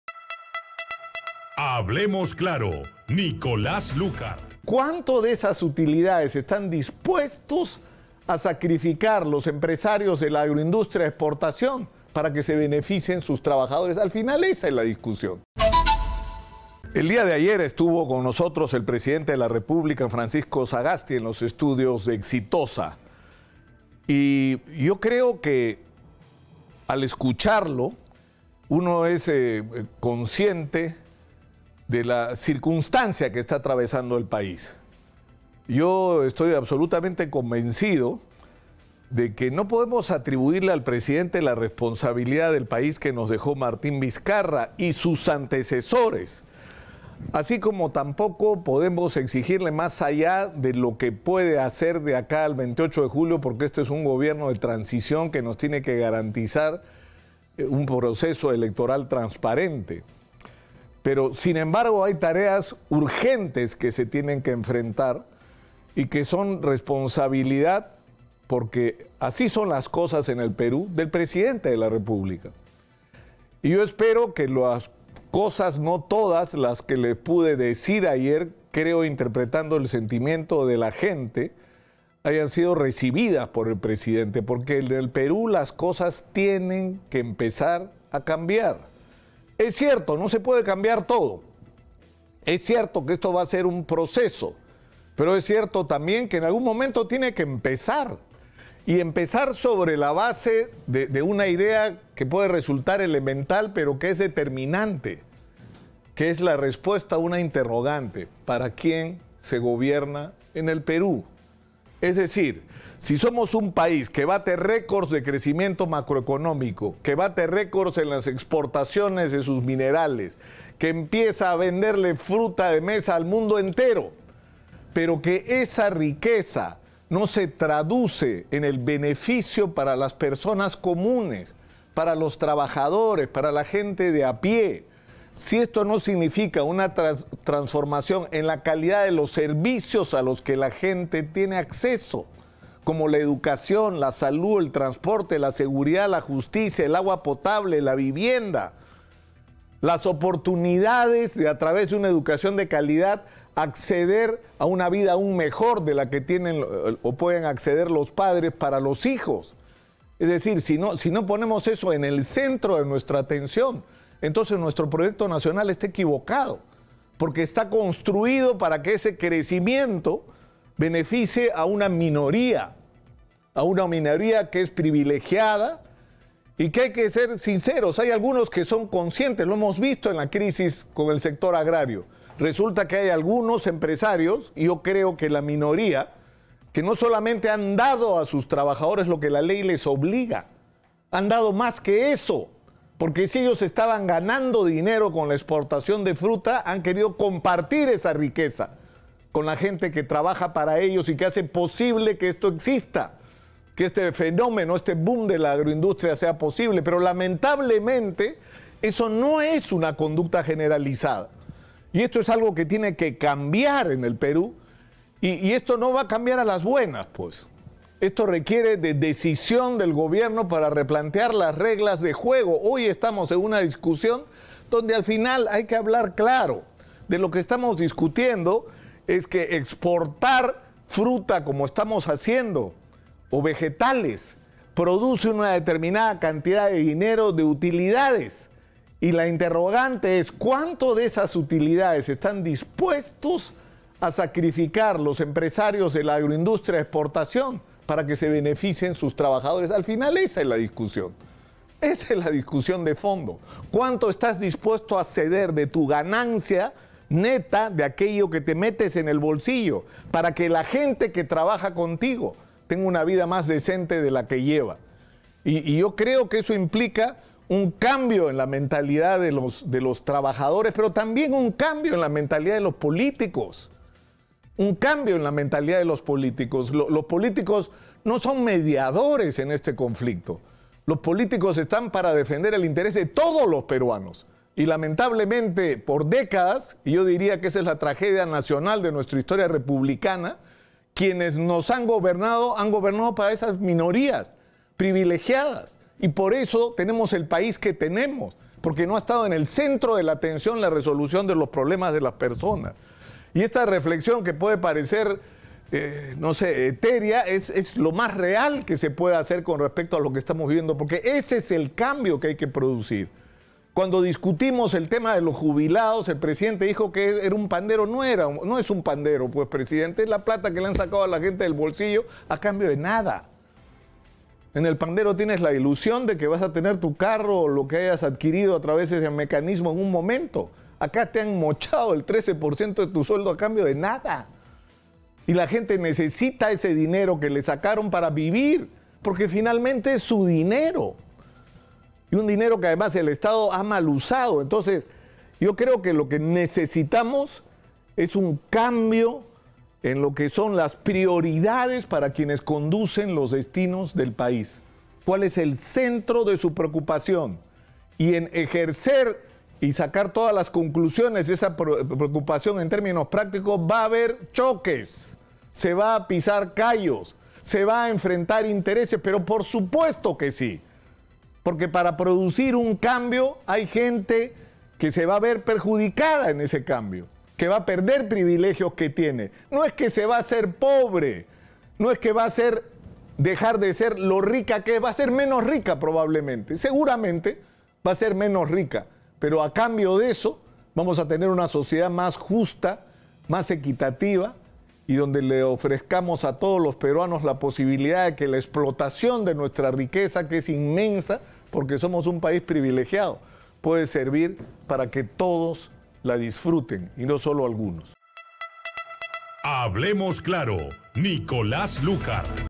🔴🔵Nicolás Lúcar pide a empresarios agrarios llegar a un acuerdo con sus trabajadores
Si los empresarios del sector agrario estaban ganando dinero con la exportación de fruta, [algunos] han querido compartir esta riqueza con la gente que trabaja para ellos y que hace posible que este fenómeno del agroindustria exista. Pero lamentablemente, no es una conducta generalizada.

Exitosa Perú
"La voz de los que no tienen voz"

Corporación Universal